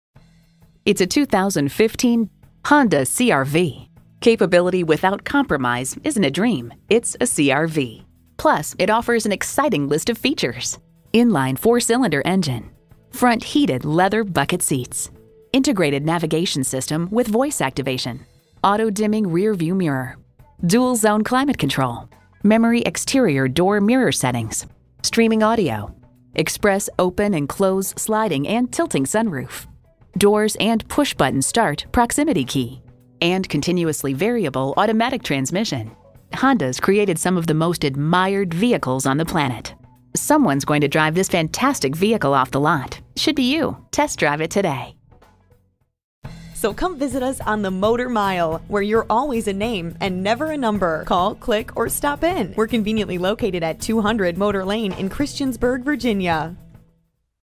Used 2015 Honda CR-V Christiansburg VA Blacksburg, VA X59380A - SOLD
Year: 2015
Make: Honda
Model: CR-V
Trim: Sport Utility
Engine: 2.4L 4 Cylinder Engine
Transmission: CVT Transmission
Color: White Diamond Pearl
Interior: Beige
Mileage: 65311
Stock #: X59380A
VIN: 5J6RM4H98FL119938